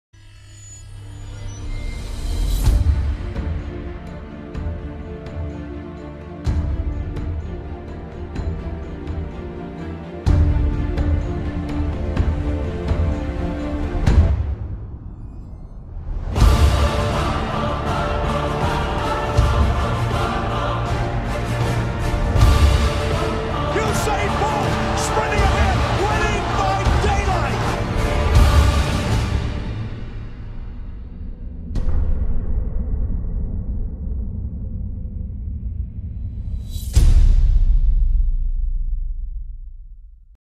Channel 24 OLYMPIC PROMO Idea,Script and Directed by Rishad Faisal for Channel 24
Channel 24 OLYMPIC-PROMO - Idea, Script and Direction: Rishad Faisal for Channel 24

Rishad Faisal
Producer,
PROMO- News and Current Affairs .